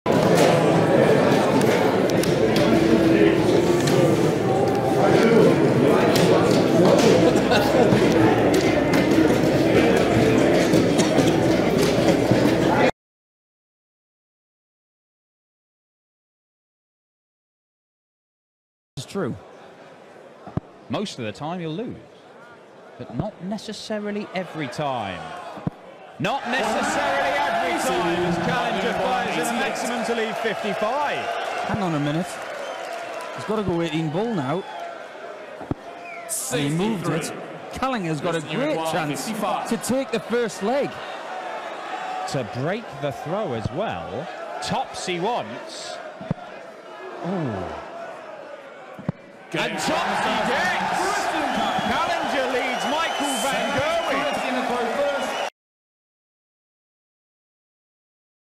Dartprofi Christian Kallinger zeigt E-Dart Trickshot Schmankerl auf der DM und ein Leg gegen MvG
Ich habe ein paar Matches von Christian Kallinger auf der DM filmen können, da er mich mit meinem Projekt paradarter.de sehr unterstützt hat und wir öfter am gleichen Ort waren.

Das Match hier wollte ich gerade zusammenschneiden, da fiel mir dieses Schmankerl auf. Das hatte ich gar nicht gesehen :P Zum Glück hat die Kamera alles eingefangen und da sieht man mal, es lohnt sich auch mehr als nur die Scheibe zu filmen.

Im Anschluss noch ein Leg von Kalli gegen Michael van Gerwen, falls jemand nicht weiß, wer Christian Kallinger ist und was er drauf hat.